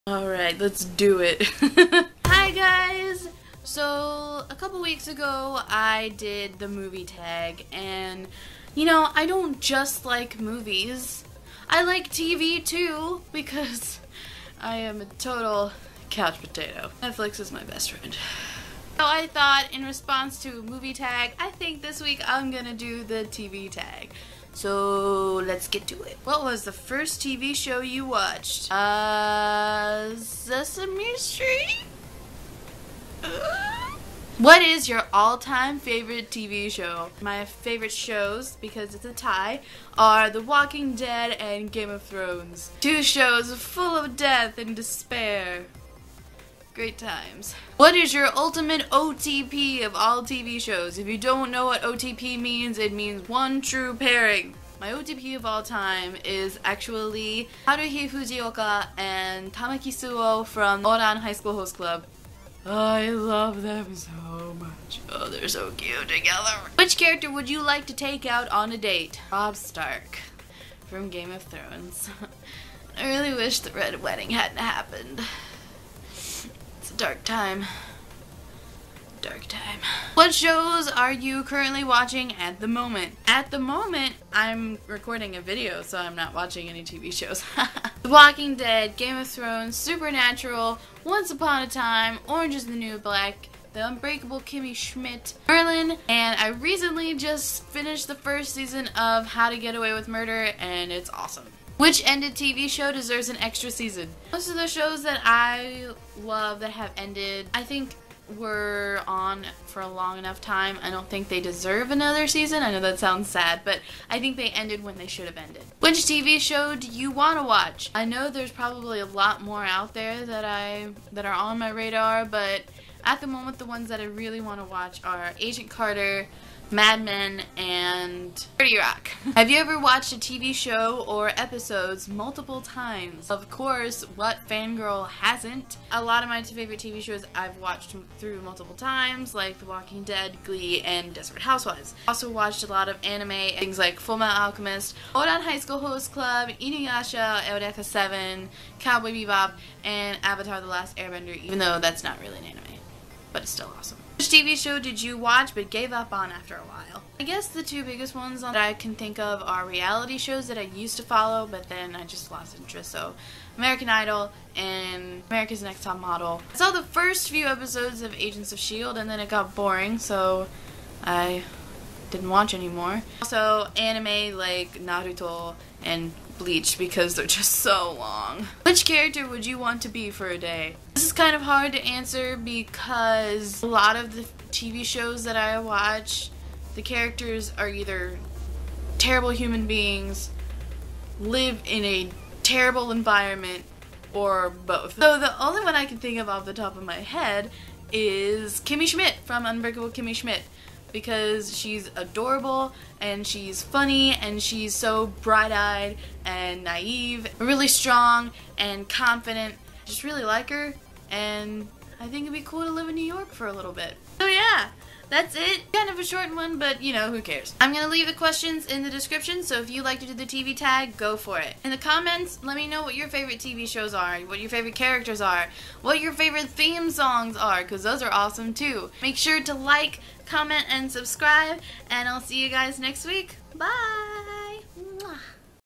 THE TV TAG
In response to the movie tag I did a couple of weeks ago, I thought I'd do the TV tag! Feel free to answer the questions yourself!

TV TAG QUESTIONS:

1. What was the first TV show you watched?

2. What is your all time favorite TV show?

3. What is your ultimate OTP?

4. Which character would you like to take out on a date?

5. Which shows are you watching at the moment?

6. Which ended TV show deserves an extra season?

7. Which TV show do you want to watch?

8. Have you ever watched a TV show (or episodes) multiple times?

9. Which TV show did you watch but gave up on after a while?

10. Which character would you want to be for a day?